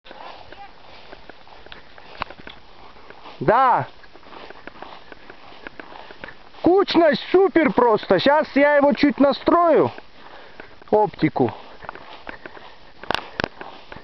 ПРИСТРЕЛКА САЙГА М3 7,62Х39 И ИЖ 18 МН 223 REM 4
СЕНТЯБРЬ. ПРИСТРЕЛИВАЕМ ПОЛУАВТОМАТИЧЕСКИЙ КАРАБИН САЙГА М3 КАЛИБРА 7,62Х39 НА БАЗЕ АК 47 ИЛИ РПК  И КАПЛАУФ ИЖ 18 МН 223 РЕМИНГТОН . ДИСТАНЦИЯ ОКОЛО 130 МЕТРОВ В ВОРОНУ НА БУМАГЕ ФОРМАТОМ Ф4 РСО-АЛАНИЯ ОСЕТИЯ ПЕРЕД ОХОТОЙ ЧЕРМЕН И ХАСАН КОЧИЕВ  УАЗ ТРОФИ РЕЙД ОХОТА SAYGA IZH 18 HUNTER ПУЛЕВАЯ СТЕНДОВАЯ СТРЕЛЬБА БЕНЧРЕСТ ВАРМИТИНГ .